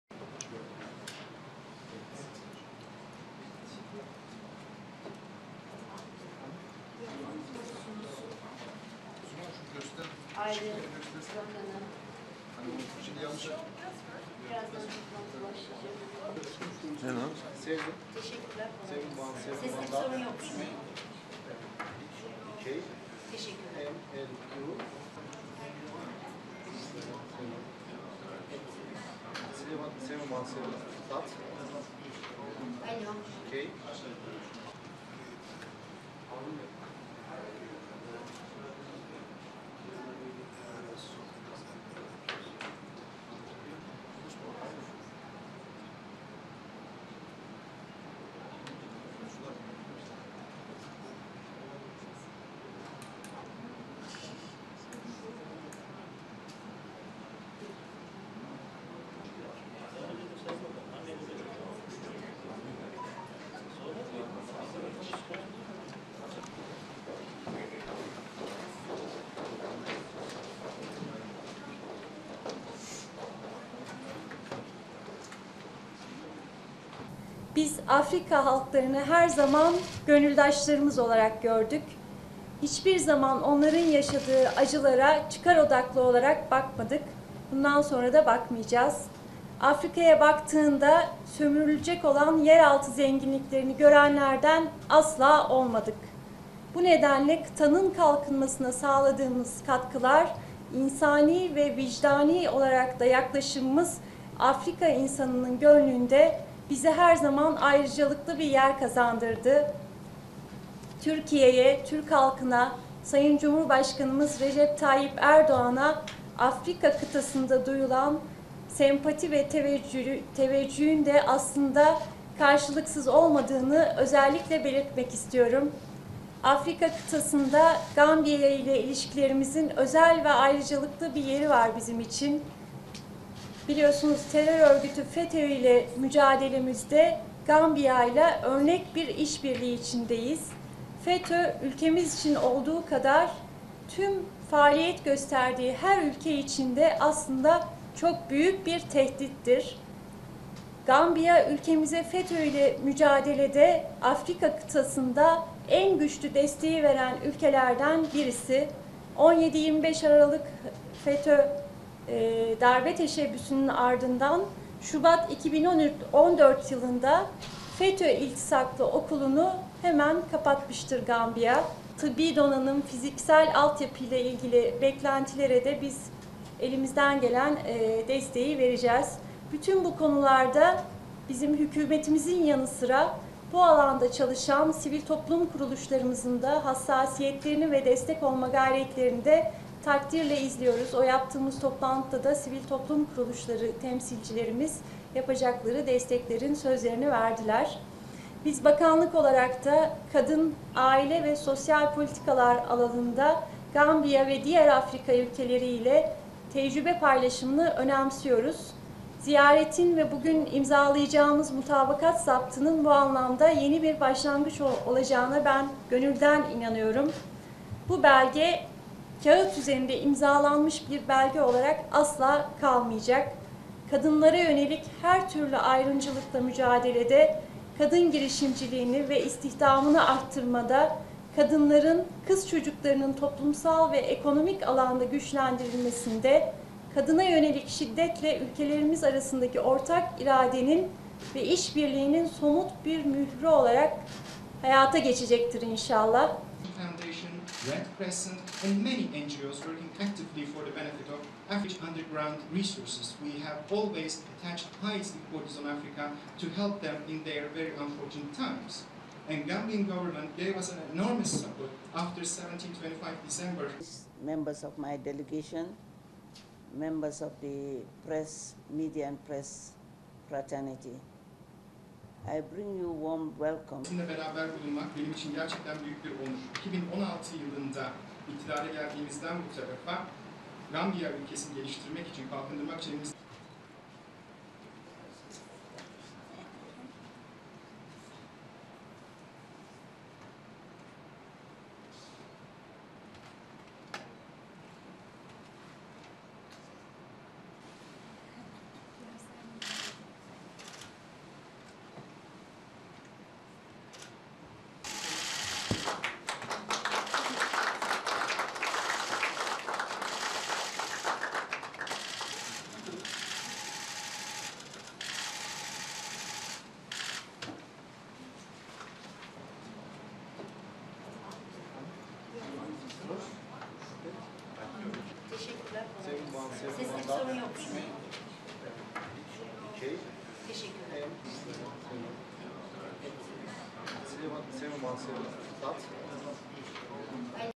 Türkiye ile Gambiya arasında iş birliği protokolü imzalandı
Aile ve Sosyal Politikalar Bakanı Fatma Betül Sayan Kaya, "Biz Afrika halklarını her zaman gönüldaşlarımız olarak gördük.